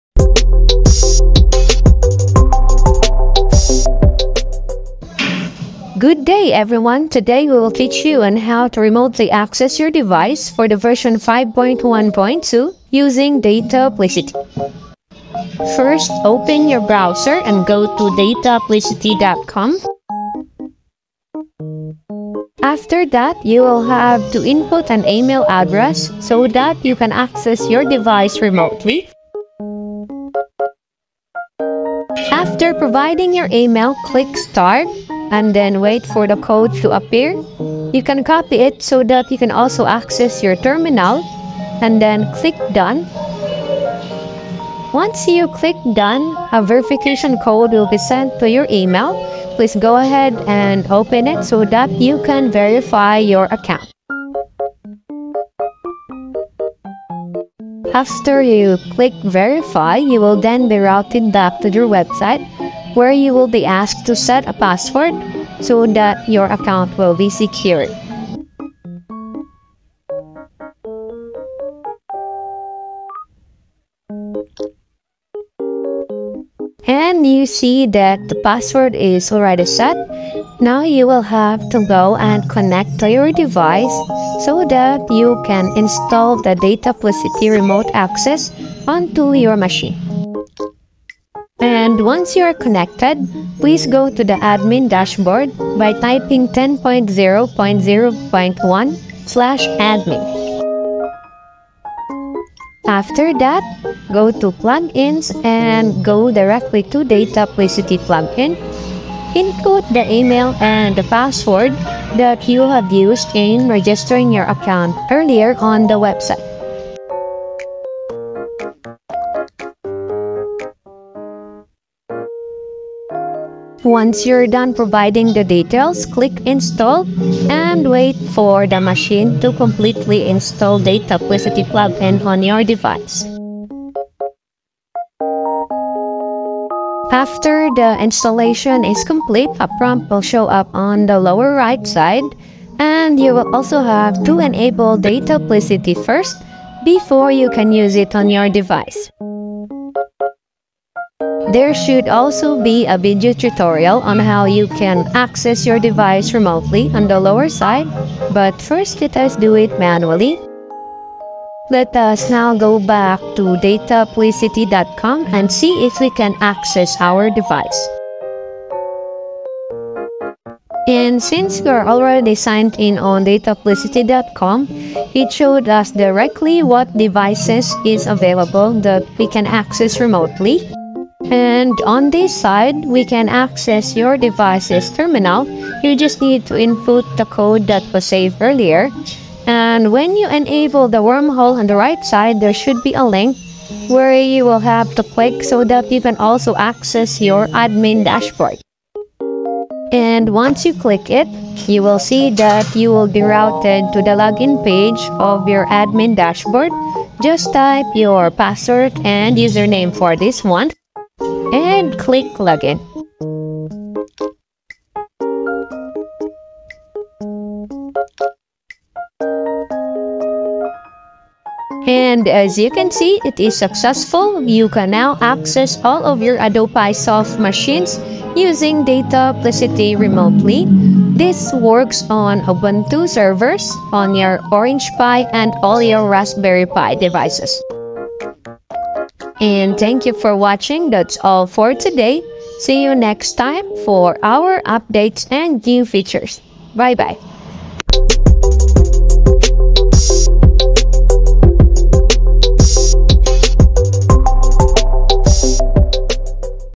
How to remote adopisoft via dataplicity version 5.1.3 to 5.1.5
paano mag access ng admin account ni adopisoft via dataplicity. eto step by step tutorial mga boss. panoorin mabuti at sundan lang mga bossing.